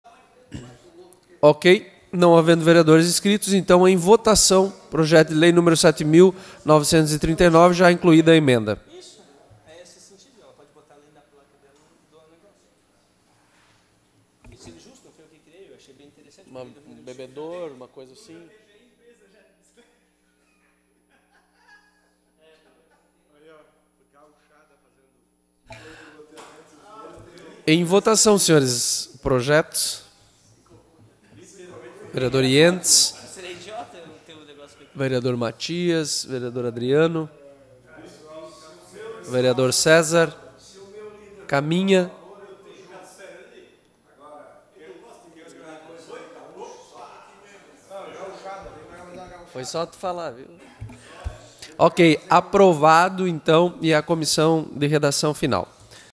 Votação Plenária - 07/11/19 - Projeto de Lei 7939/2019
Projeto de Lei 7939/2019, de autoria do vereador Bruno Cunha, que "CRIA O PROGRAMA ABRACE UMA CICLOVIA NO MUNICÍPIO DE BLUMENAU.", com a emenda Nº 01 incorporada. A emenda substitutiva altera as redações do § 1º do artigo 2º e do artigo 3º, para os seguintes termos: § 1º Os parceiros referidos no caput deste artigo poderão afixar nos equipamentos adotados na forma desta lei, em local visível, peças publicitárias que contenham seu nome e sua logomarca. Art. 3º O Programa Abrace uma Ciclovia permitirá às empresas privadas implantar, nas ciclovias ou ciclofaixas, às suas expensas, estações para oferecer auxílio técnico aos usuários, reparos de bicicletas e bicicletários.